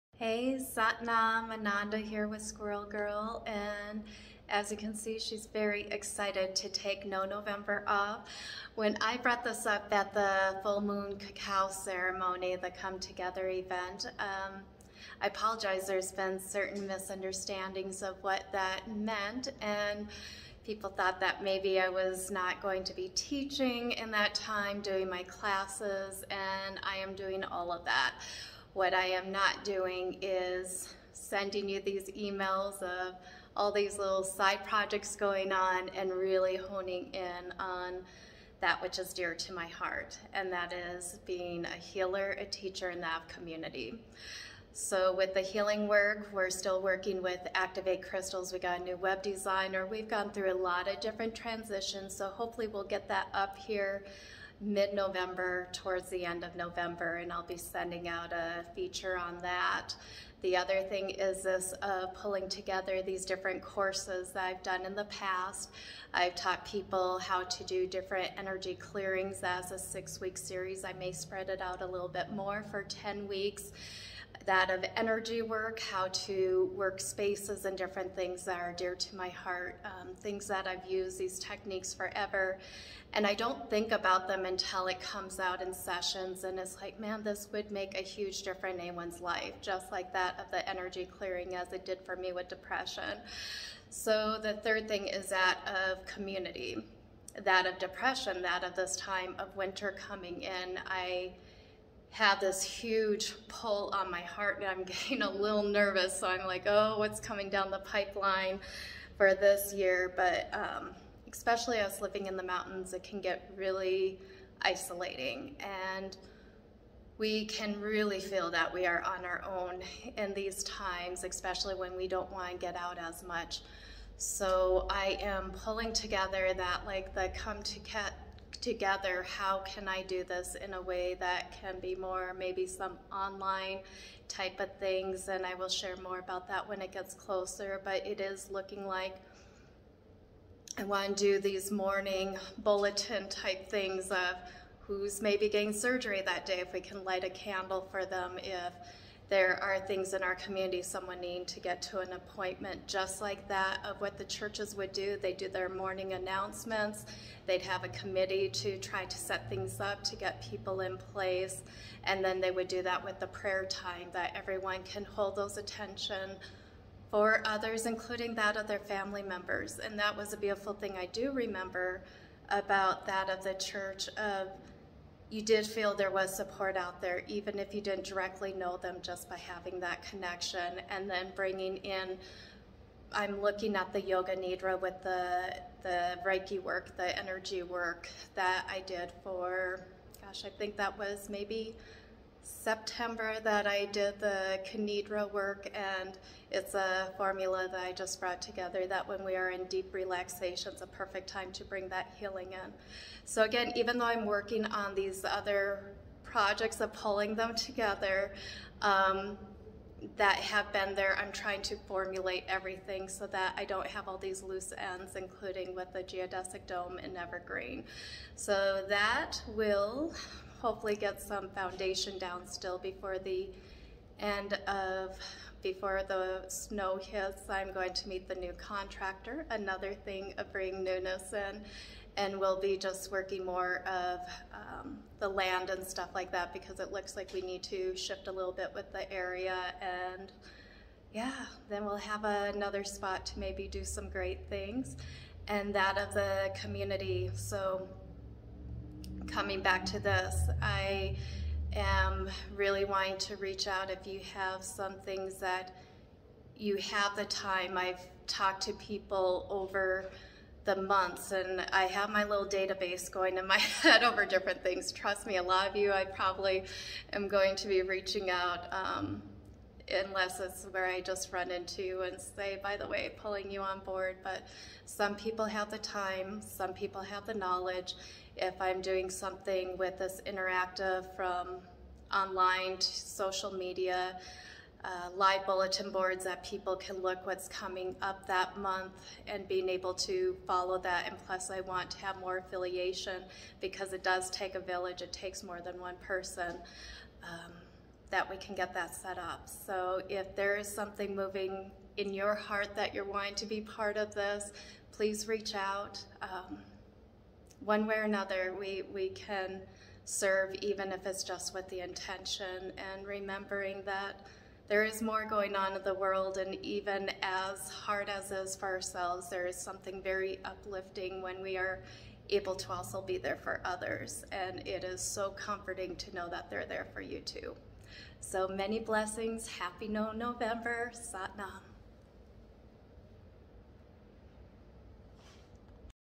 SquirrelGirl on vaca for "No November"- Ananda is still here
November update what's the intent- Healing, Teaching, Community. Please reach out if you wish to get involved Speerlight.com See schedule and book appointments through taspenshealingcenter.com